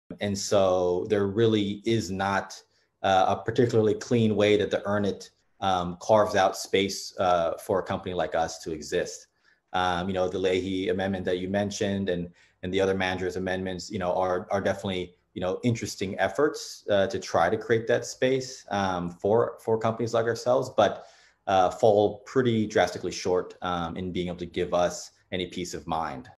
Fight for the Future EARN IT Act livestream: Jun Harada clip 5
- Fight for the Future spoke with Jun Harada, Head of Growth and Communication at Signal, about the EARN IT Act.  Here's what he had to say about this dangerous new law that threatens free speech and privacy on the Internet